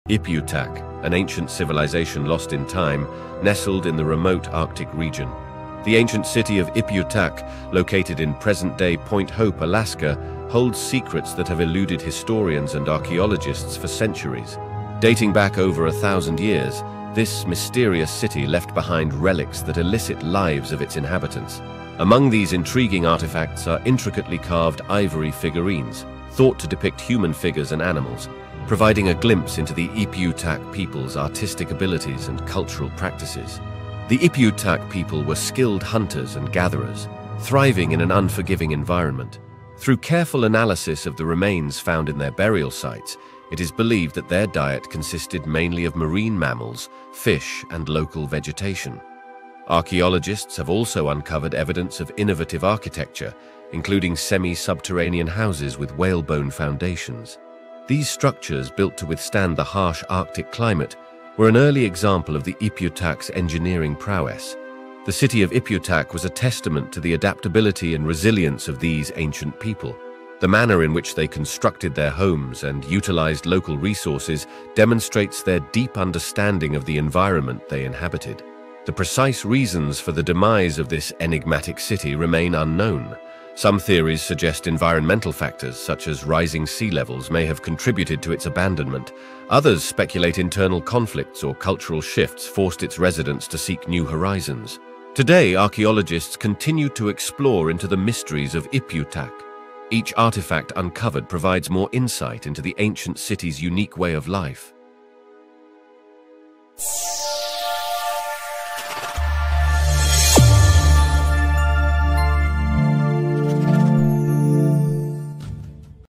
The Lost Ancient City of Ipiutak
Located at Point Hope, Alaska, the ruins of Ipiutak offer a glimpse into the past when the city was alive and bustling. Though only ancient artifacts remain, the archaeological and historical value of the site remains immense. The most fascinating part of this site is the unknown origin of the city's builders.

Copyright Information

Images: Wikimedia Commons, Public Domains, Pixabay, Stock (Commercial)

Video Clips: Stock (Commercial), Wikimedia Commons, Public Domains